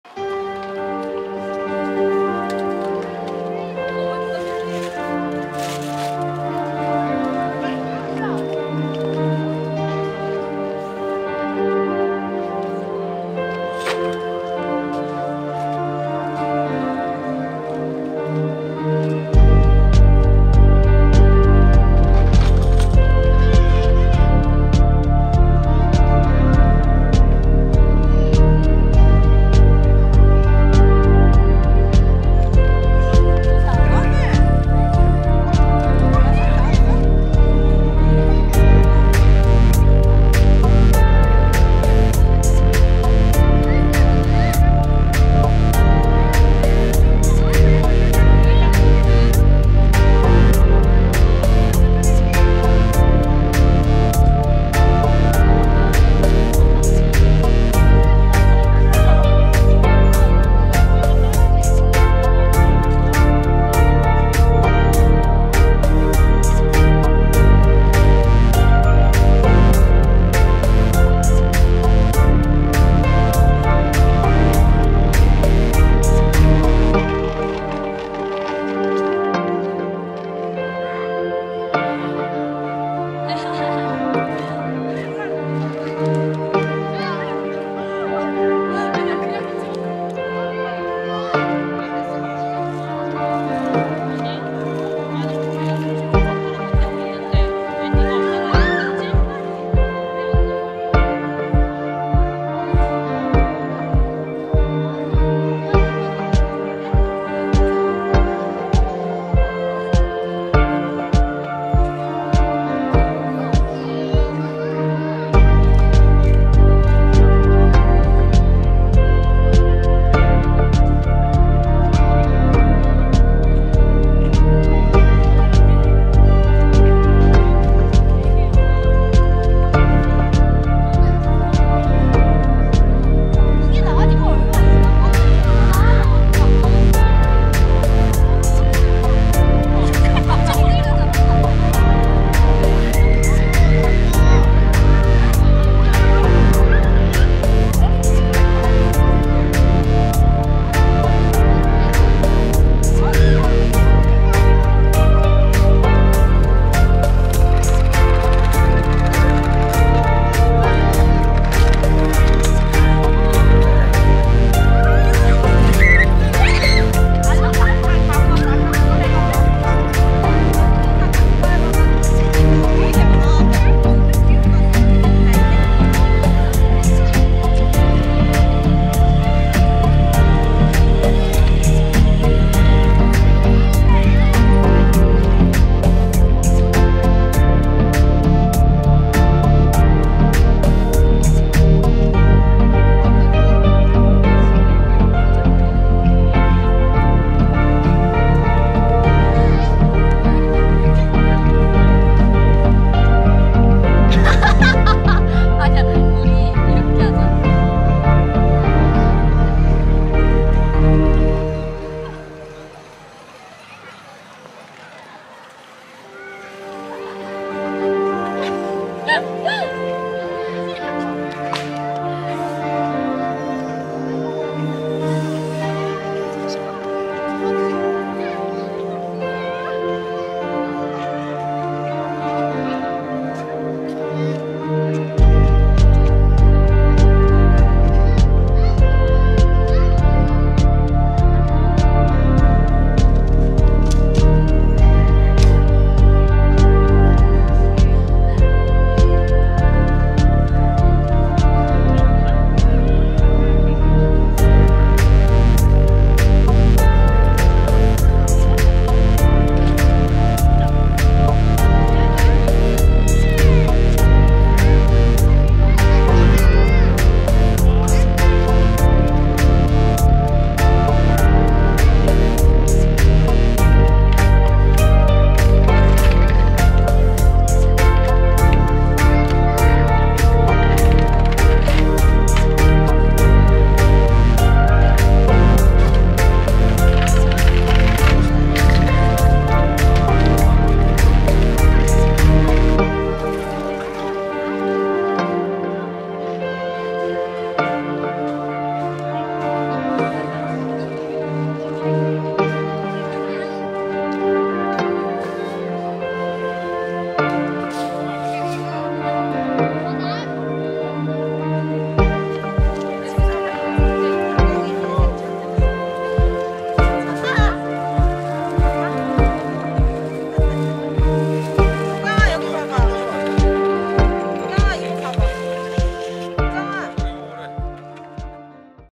평촌중앙공원에 활짝 핀 튤립
평촌중앙공원에 튤립이 예쁘게 피어 있군요. 안구 정화하고 가세요!! [배상복TV]